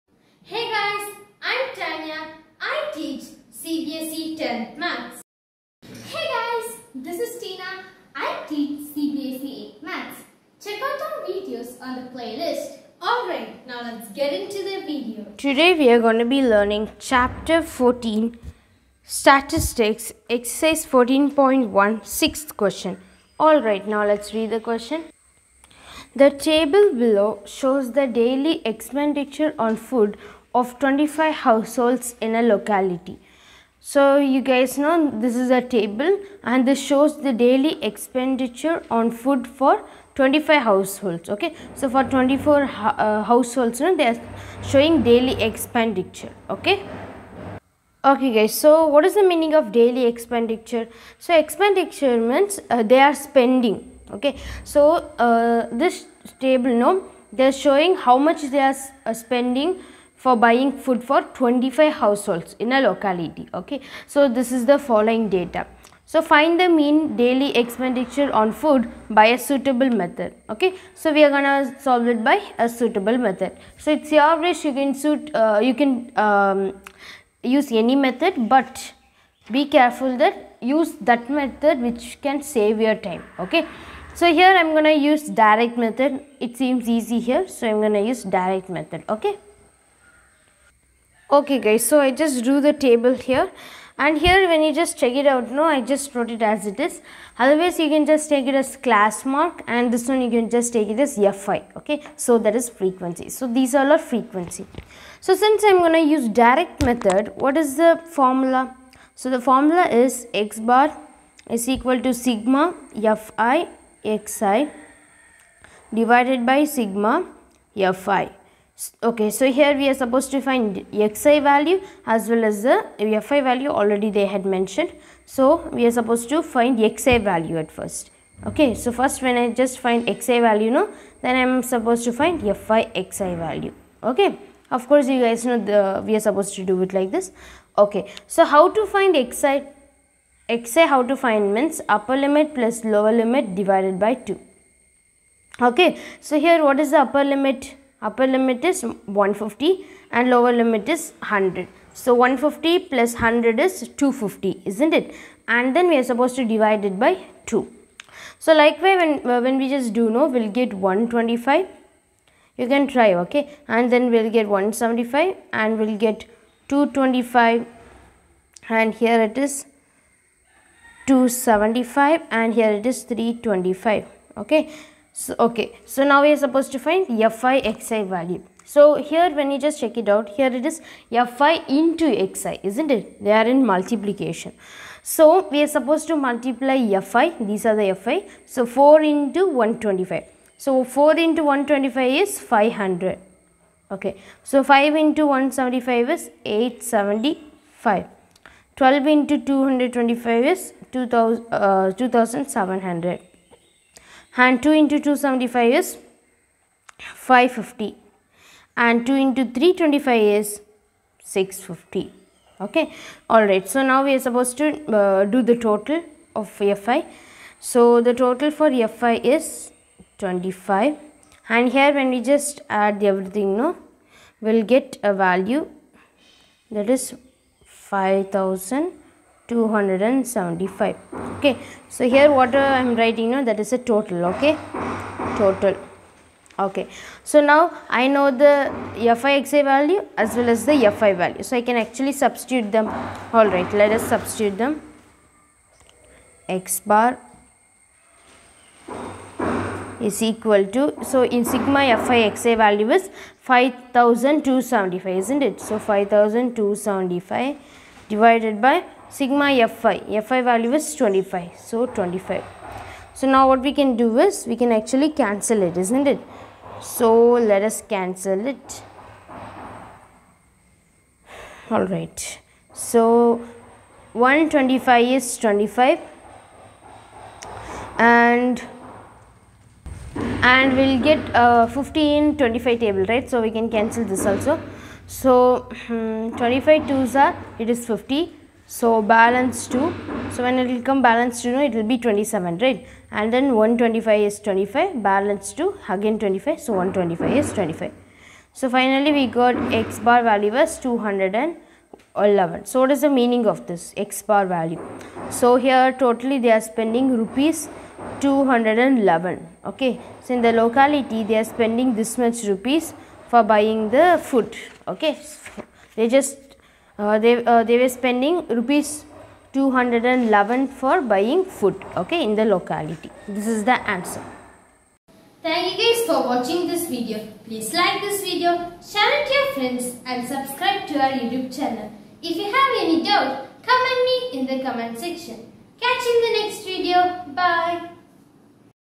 Ex 14.1 q6 Chapter 14 statistics class 10 math NCERT Syllabus
This video is about Statistics. In this video we solved Exercise 14.1 q6 and we also learned about the basic things about Statistics.This is really  easy to solve if you guys know the basic things of this particular chapter Statistics. Statistics fully based on the  formulas .We have to use our thinking and solve the questions based on Statistics

Try to understand the basics of Statistics that you guys can solve any questions based on this Statistics. If you understand properly it will be easy to solve questions based on Statistics.